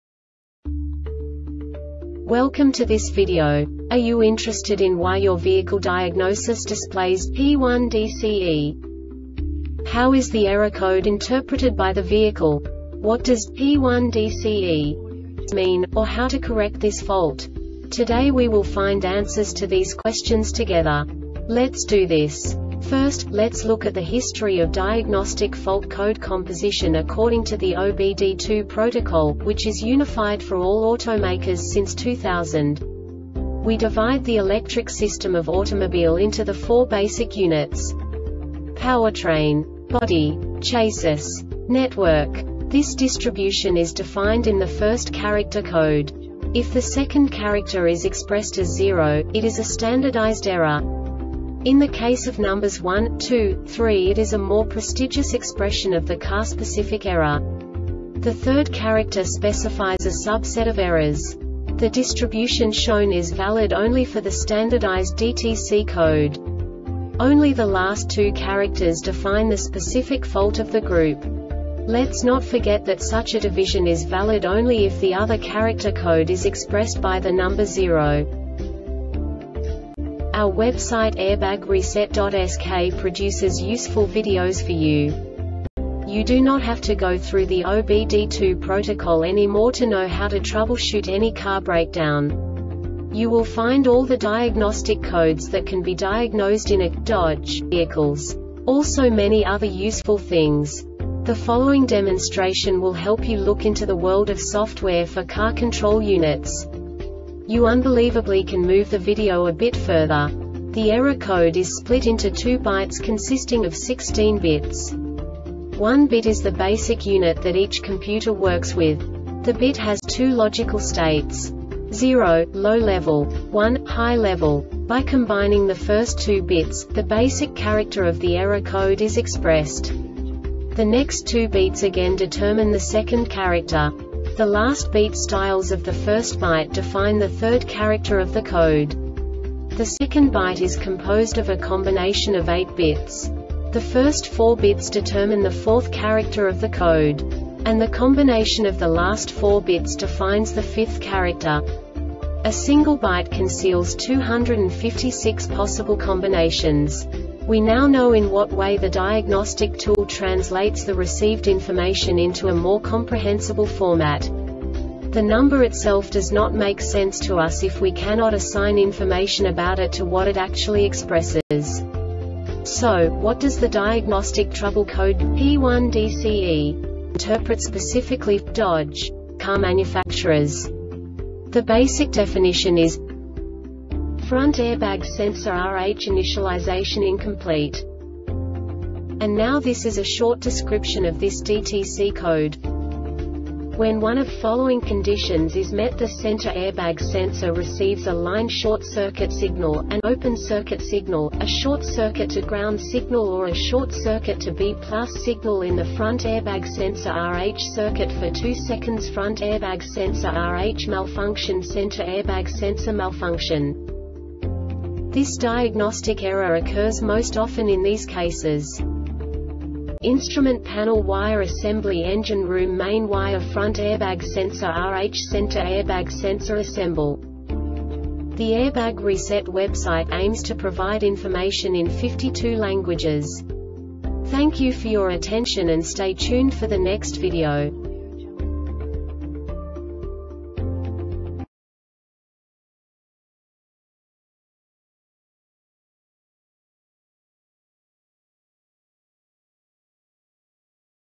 DTC Dodge P1DCE Short Explanation
Contents:
0:21 Basic DTC analysis according to OBD2 protocol standard.
1:48 Insight into programming
2:58 A diagnostically understandable description of the Diagnostic Trouble Code
3:05 Basic error definition

Make:
Dodge
Code:
P1DCE
Definition:
Front Airbag Sensor RH Initialization Incomplete
Description:
When one of following conditions is met:The center airbag sensor receives a line short circuit signal,an open circuit signal,a short circuit to ground signal or a short circuit to B+ signal in the front airbag sensor RH circuit for 2 seconds. Front airbag sensor RH malfunctionCenter airbag sensor malfunction
Cause:
 Instrument panel wire assembly Engine room main wire Front airbag sensor RH Center airbag sensor assembl